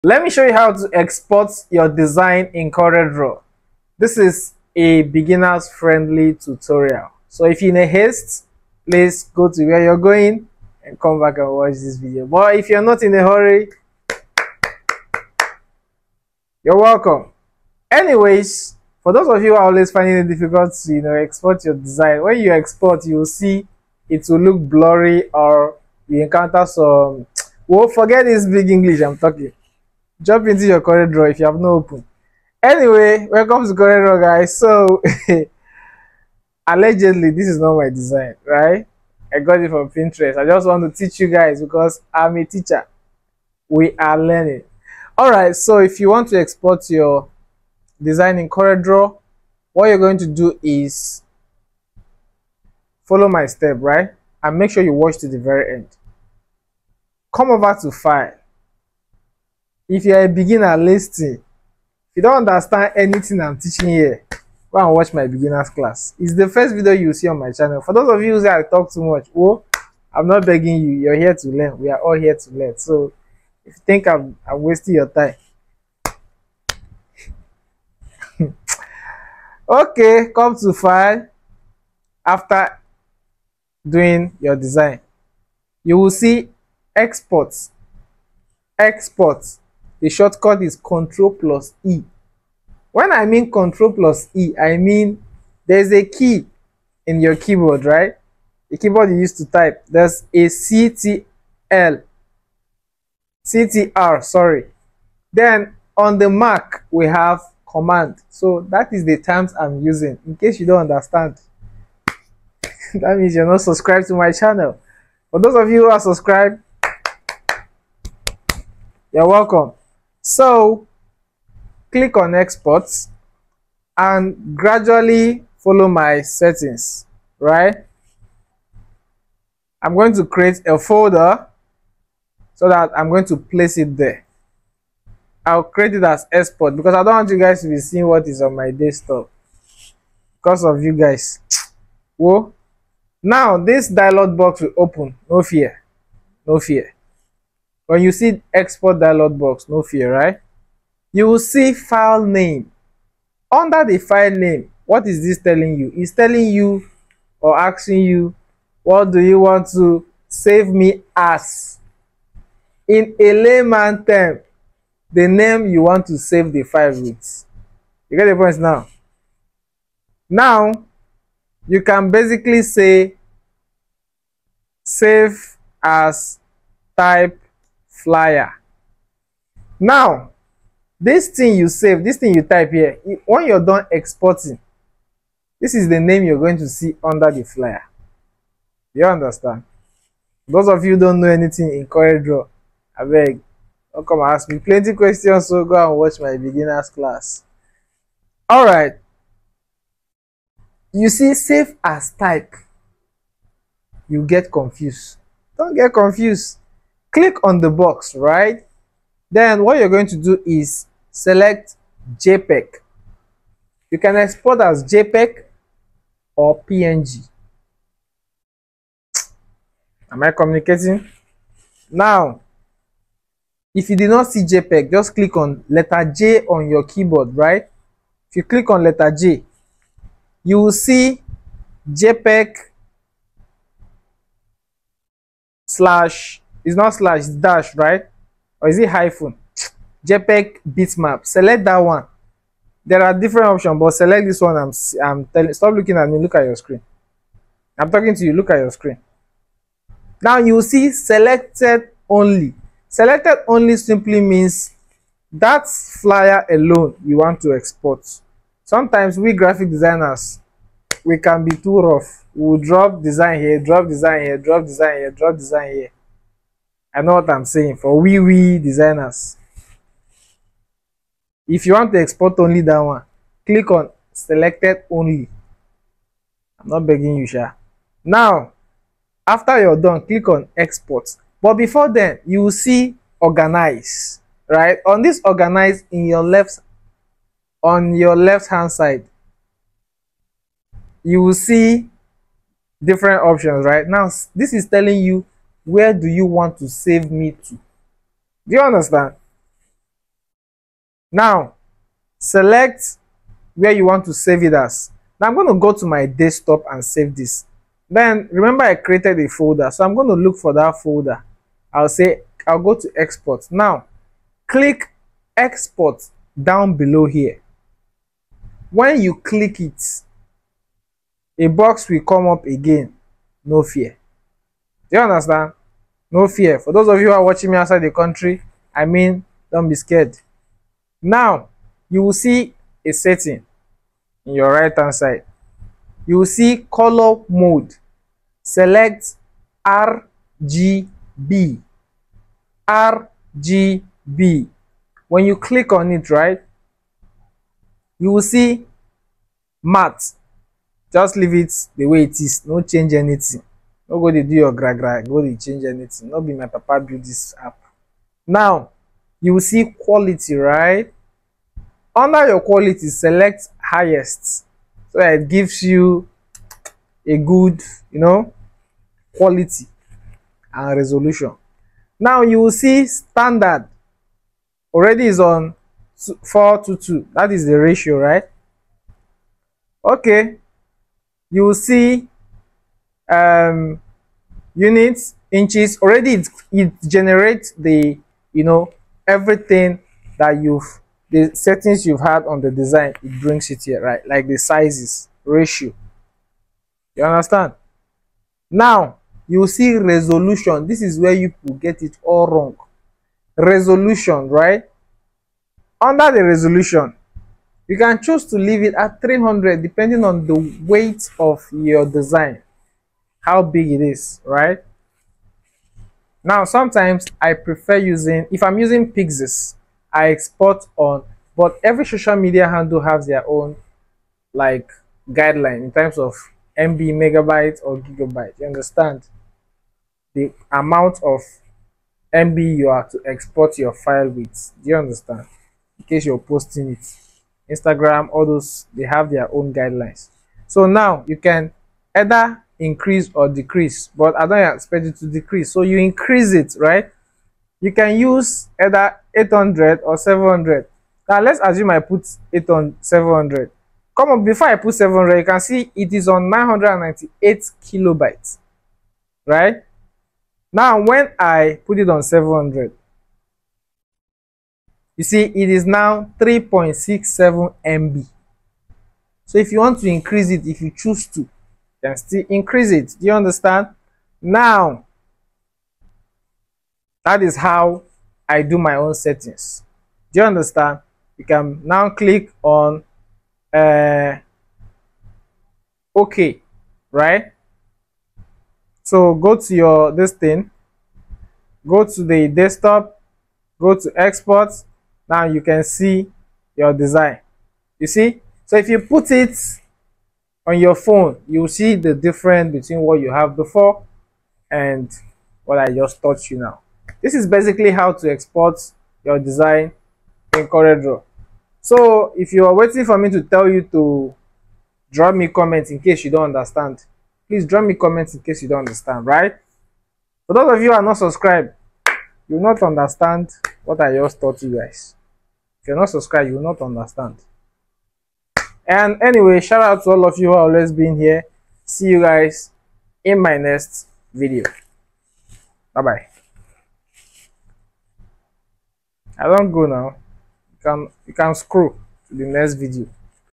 How to Export Design in CorelDraw | Beginner Tutorial
In this video, I will teach you how to  export design in coreldraw

H  O  W   T O        R  E  M  O  V  E      B  A  C  K  G  R  O  U  N  D

D O N T   M  I  S  S    O  U  T